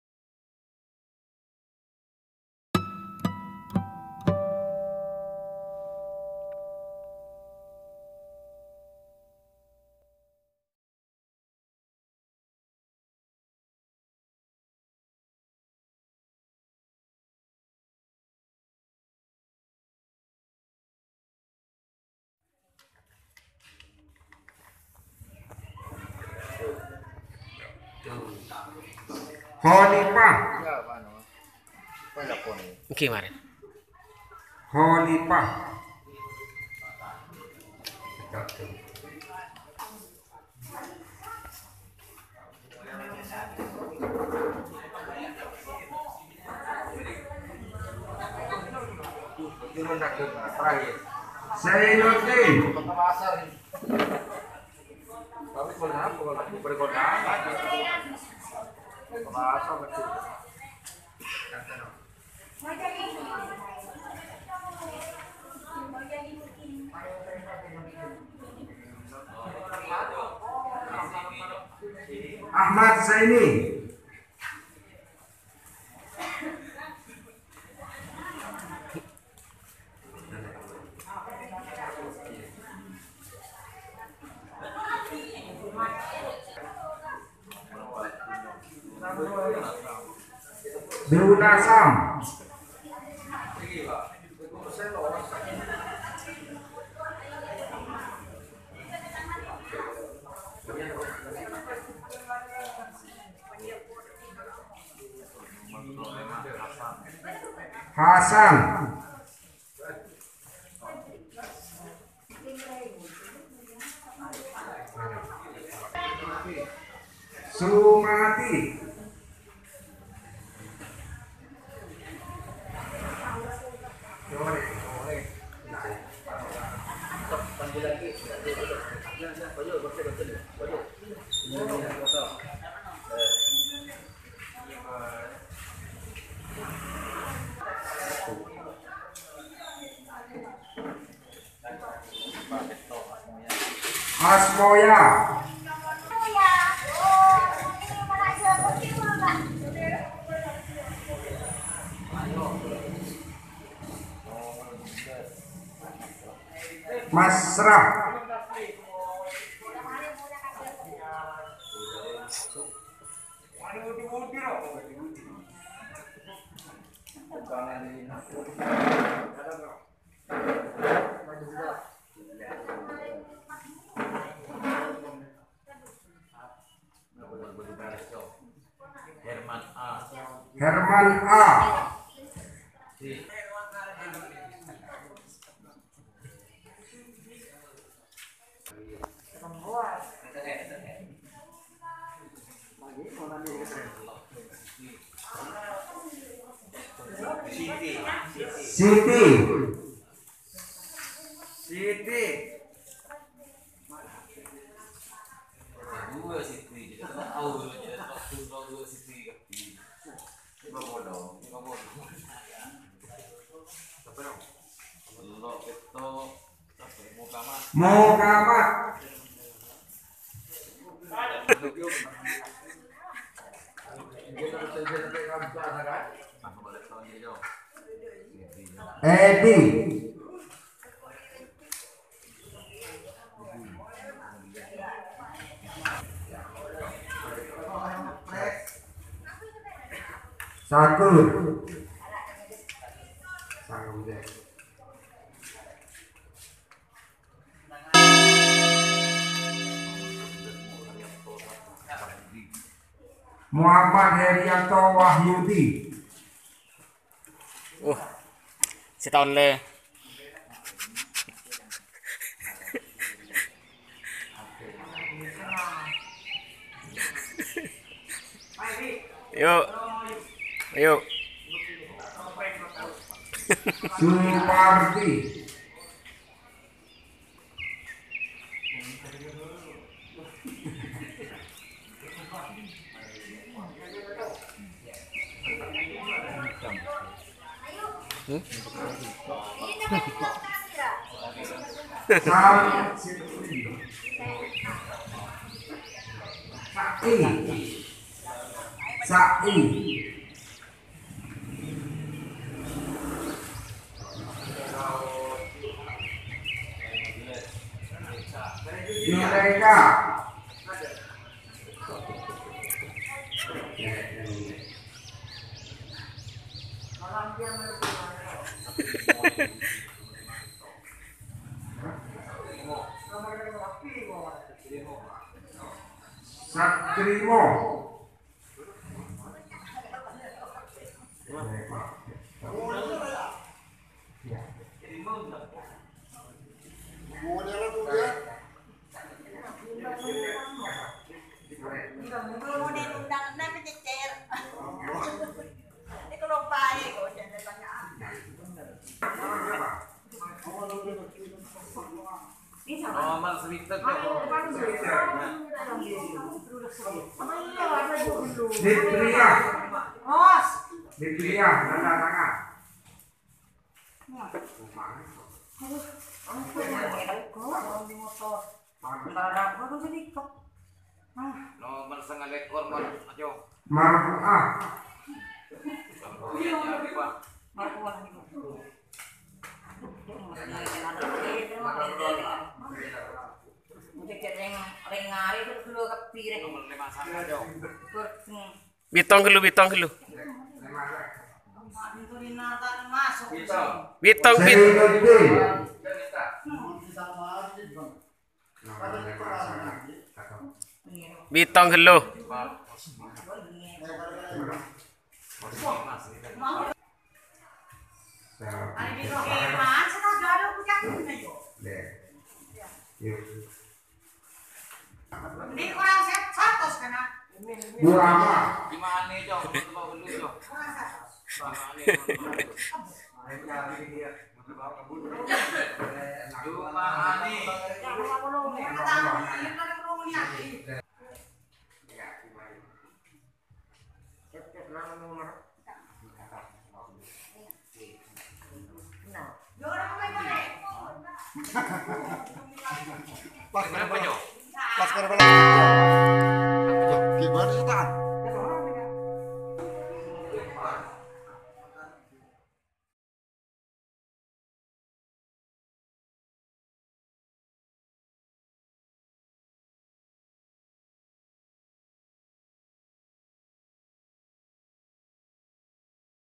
BLT DD 1 Sudah Tersalurkan Desa Tampojung Guwa || TV Desa
BLT DD Tahap 1 Sudah tersalurkan di Desa Tampojung Guwa Waru Pamekasan Madura Jawa Timur.